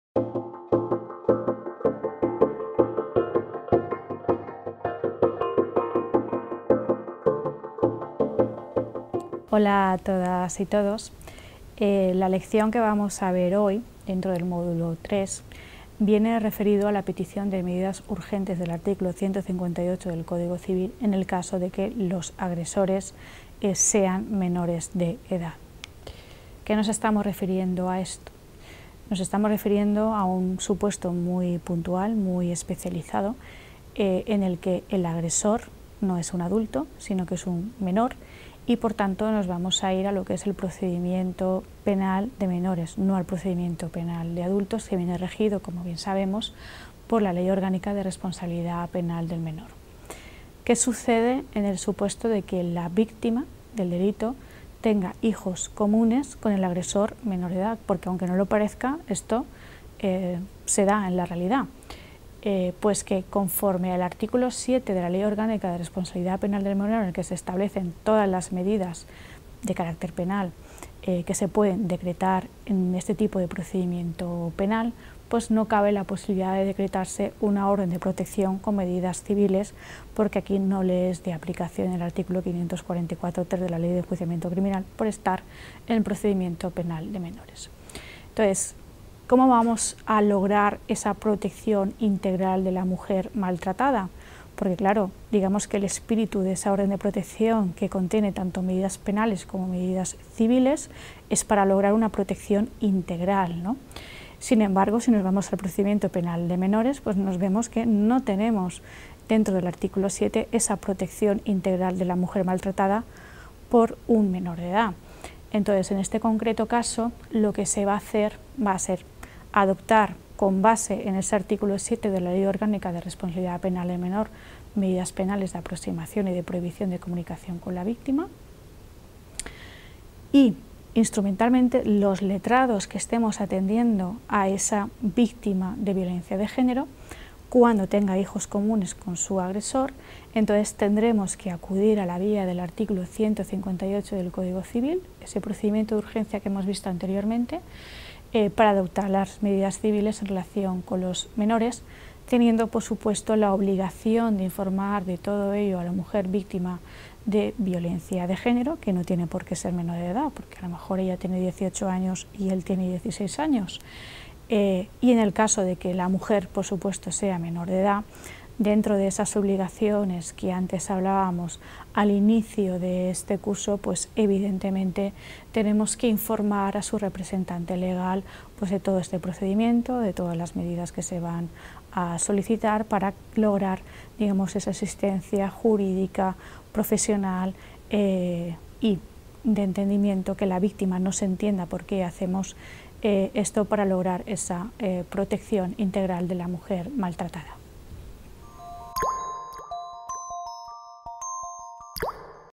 5.3.4. Petición de medidas urgentes del art. 158 CC en casos de agresores menores de edad
Autora: Jordán Díaz-Roncero, Mª José.
Curso: Formación básica en materia de violencia de género.
Resumen: En esta lección analizamos la actuación letrada en los supuestos de que el agresor sea un menor y estemos en presencia de un procedimiento penal de menores.
Año: 2019.